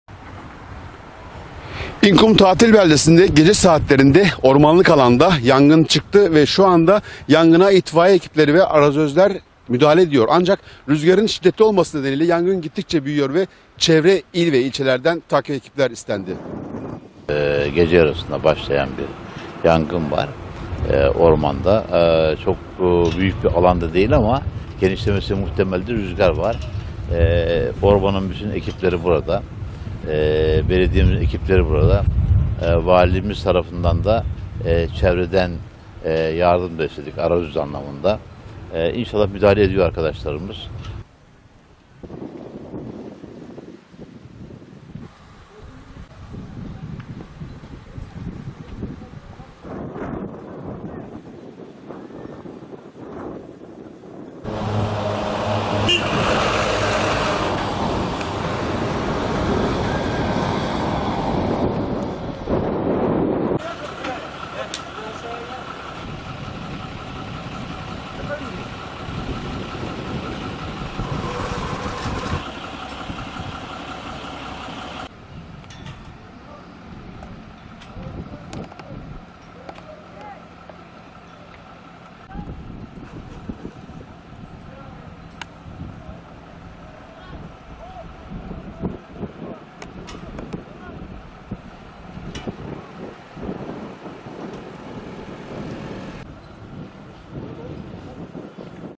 İnkumu'nda yangın çıktı
BARTIN’ın İnkum tatil beldesinde gece saatlerinde Bartın’ın İnkum tatil beldesi ormanlık alanda bilinmeyen bir nedenle yangın çıktı. Yangını gören vatandaşlar durumu itfaiye ekiplerine haber verdi. Rüzgarın da etkisiyle kısa sürede büyüyen yangına Orman İşletmesi'nin yanı sıra itfaiye ekipleri de müdahale etti. Sarp kayalık arazide yayılan alevler evlere yaklaşık 100 metre kala devam ediyor.